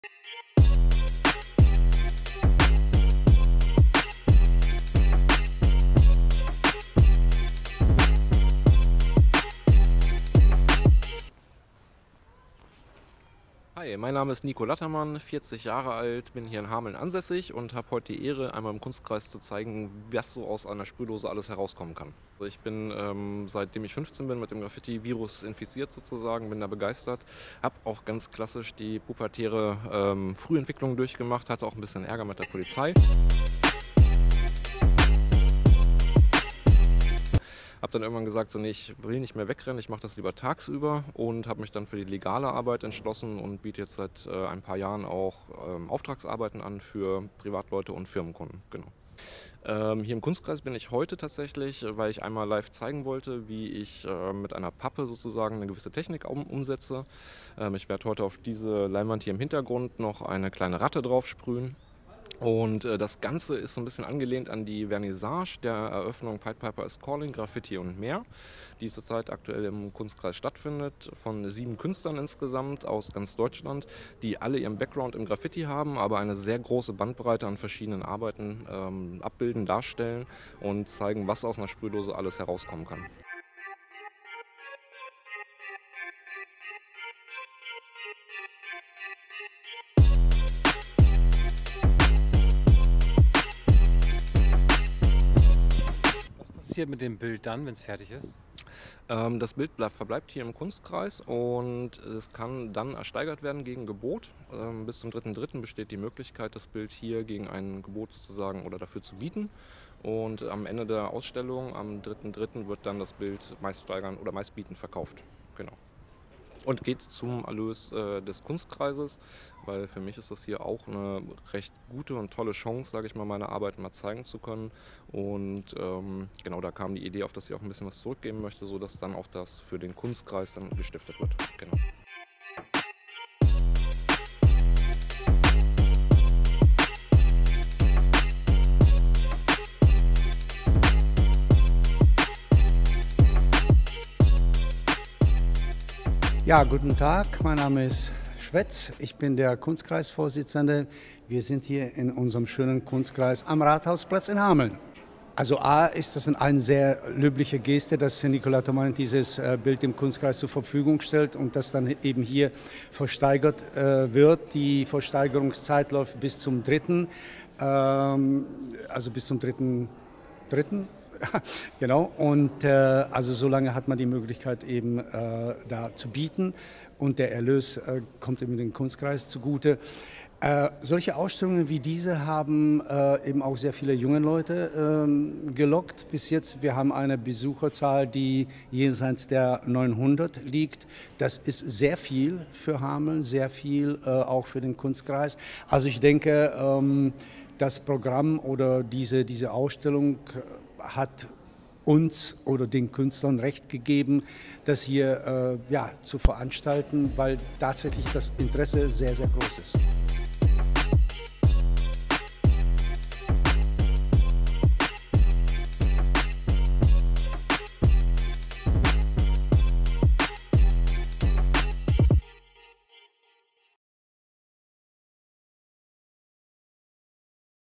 Legal sprühen: Graffiti kommt in Hameln sehr gut an!
Besucherrekord bei Graffiti-Ausstellung in Hameln. Der Kunstkreis freut sich über das große Interesse an der aktuellen Ausstellung. Kurator Nico Lattermann alias Shaz zeigte am Wochenende seine Kunst live vor Publikum und sprühte im Hof des Kunstkreises ein Rattenfänger-Bild.